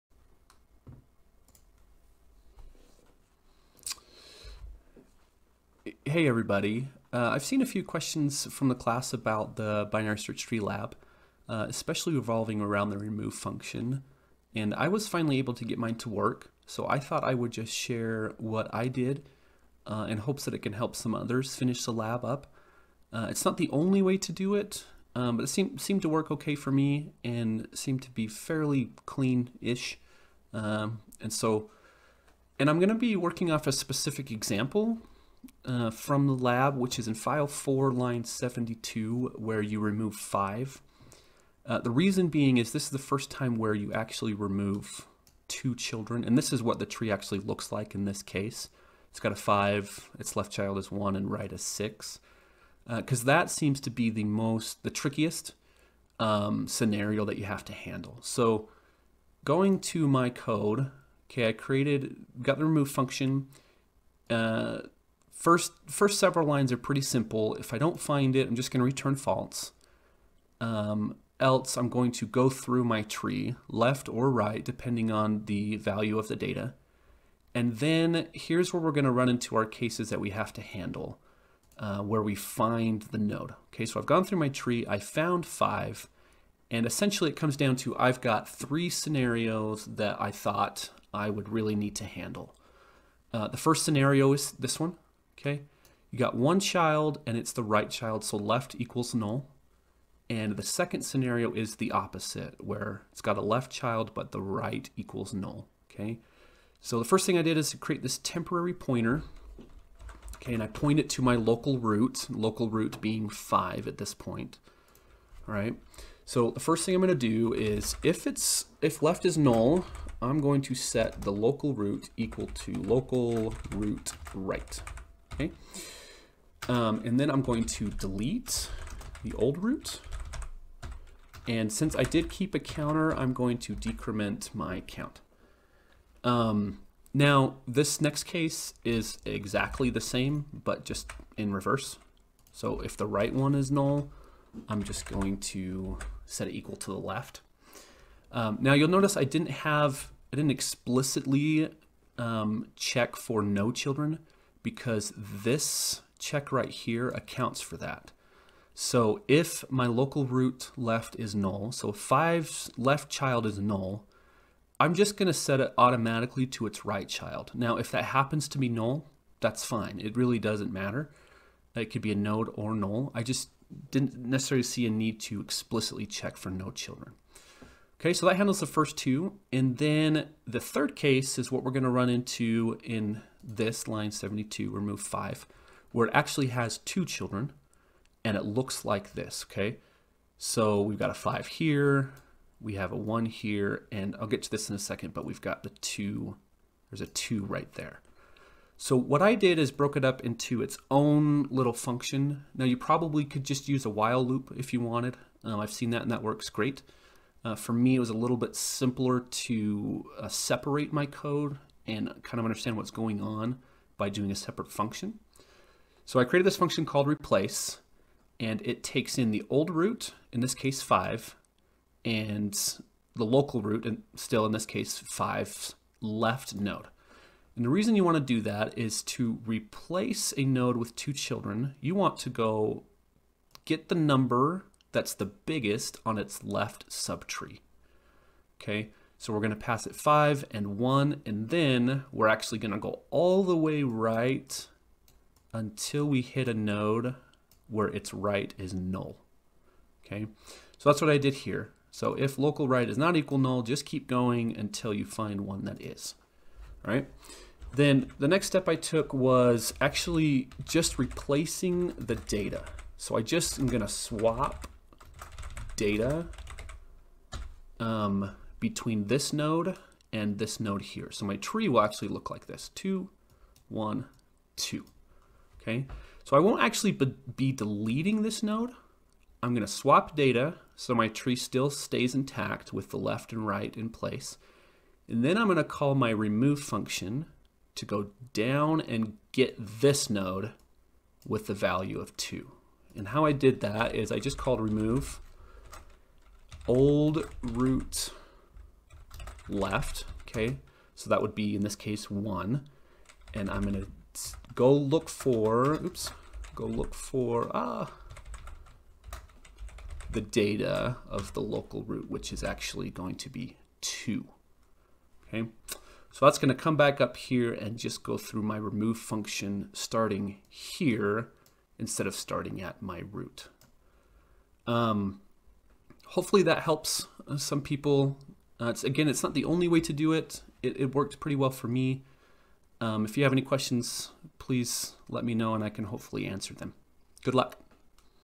BST Remove
This video goes through how to remove a node from the BST